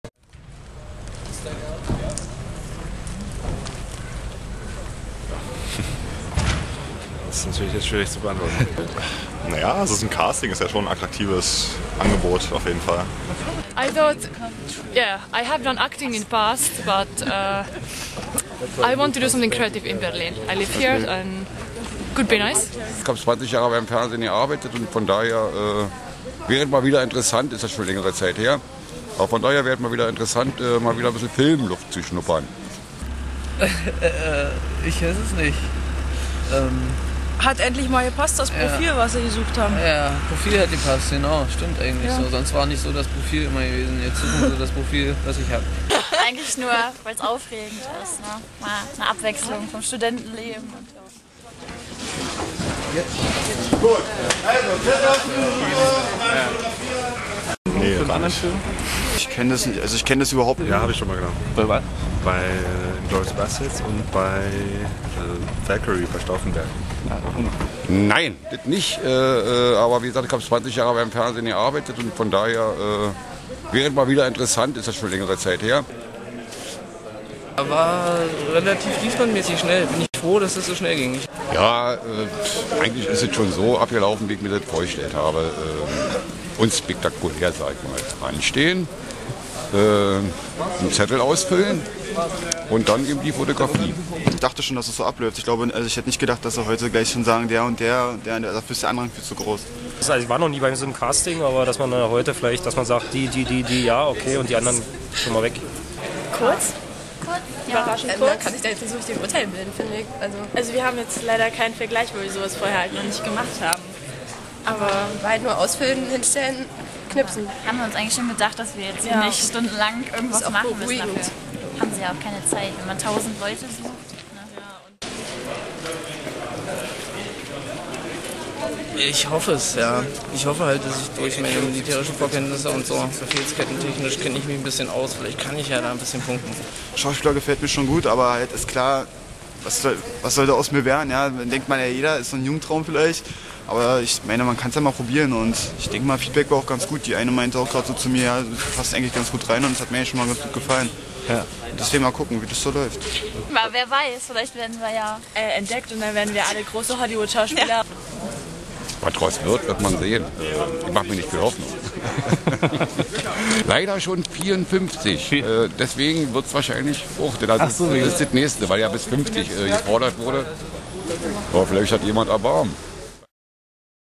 Casting für neuen George-Clooney-Film in Babelsberg
Am Samstag, 26. Januar 2013, wurden weit über 3.000 Interessierte für den neuen Film von und mit George Clooney "The Monuments Men" von der Agentur Filmgesichter gecastet. Bei klirrender Kälte standen die Menschen in langen Schlangen an. MAZonline hat sie begleitet und Eindrücke vor, während und nach dem Casting gesammelt.